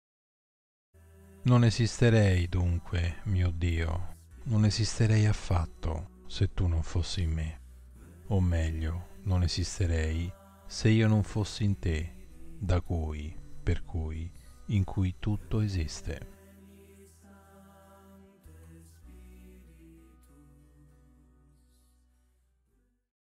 Le Parole dei Santi per oggi, 8 Maggio 2022 Sant'Agostino
"Non esisterei, dunque, mio Dio, non esisterei affatto, se tu non fossi in me. O meglio non esisterei se io non fossi in te, da cui, per cui, in cui tutto esiste".

"I would not exist, therefore, my God, I would not exist at all, if you were not in me. Or rather, I would not exist if I were not in you, from whom, in whom everything exists."

" Je n'existerais donc pas, mon Dieu, je n'existerais pas du tout, si vous n'étiez pas en moi. Ou plutôt, je n'existerais pas si je n'étais pas en toi, de qui, en qui tout existe."

"No existiría, pues, Dios mío, no existiría en absoluto, si tú no estuvieras en mí. O mejor dicho, no existiría si no estuviera en ti, de quien, en quien todo existe'.

"Eu não existiria, portanto, meu Deus, eu não existiria de todo, se não estivésseis em mim. Ou melhor, eu não existiria se não estivesse em vós, de quem, em quem tudo existe".